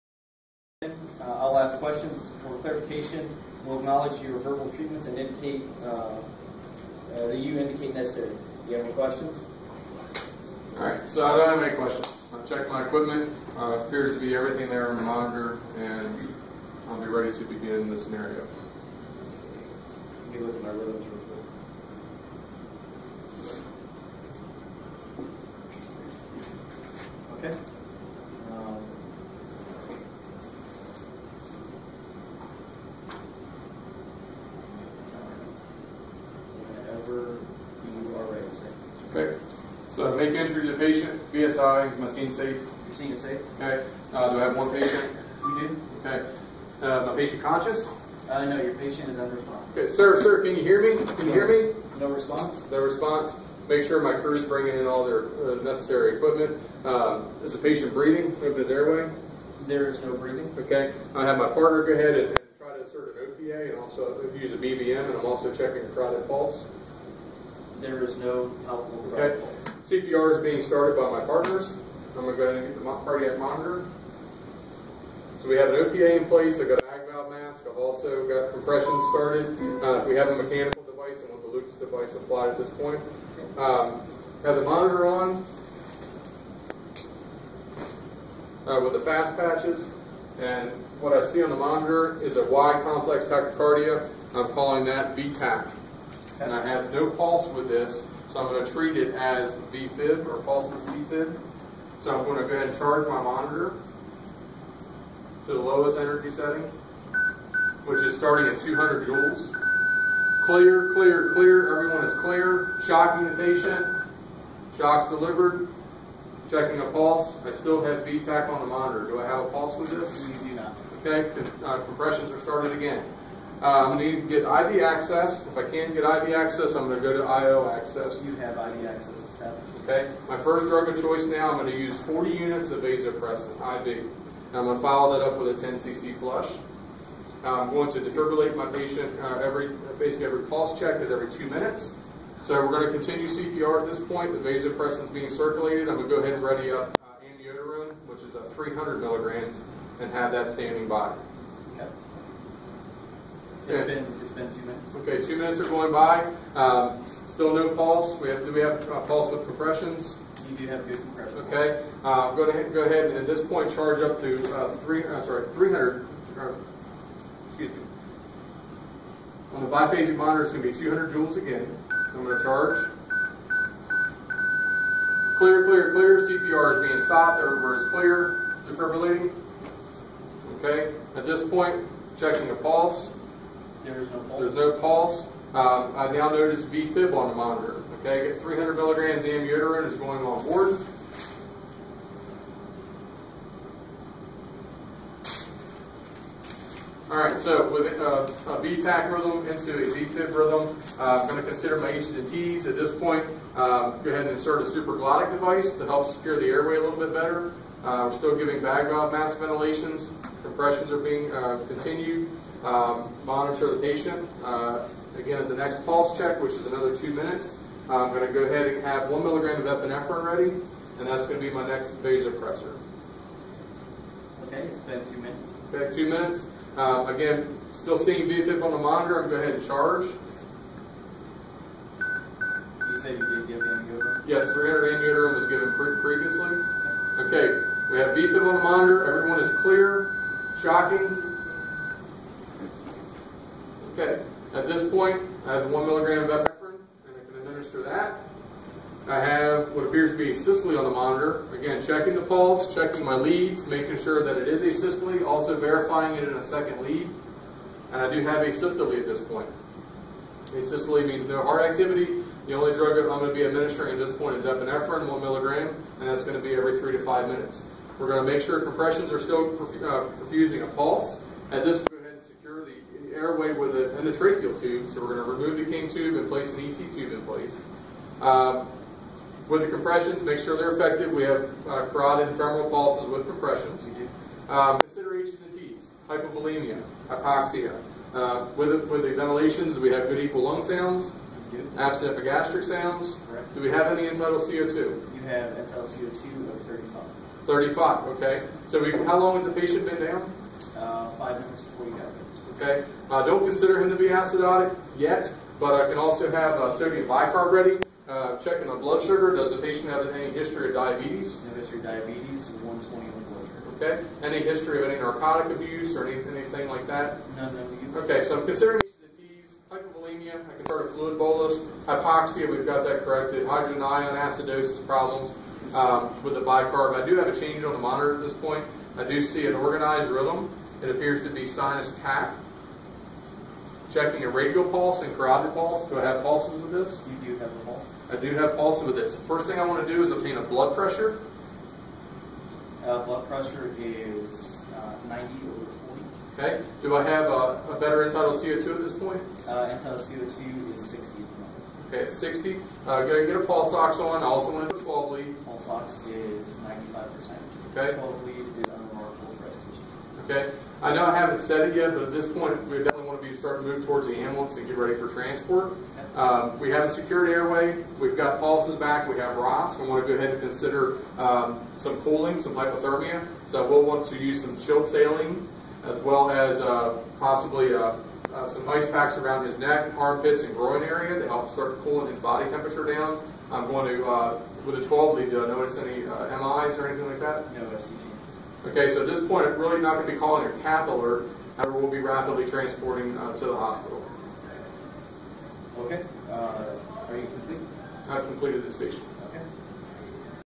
Dynamic Cardiology Mega Code
Graig Temple, Clinical Instructor with CCEMS, demonstrating the ACLS Mega Code for National Registry of Emergency Medical Technicians Advanced Level Psychomotor Examination for "DYNAMIC CARDIOLOGY" Practice Session.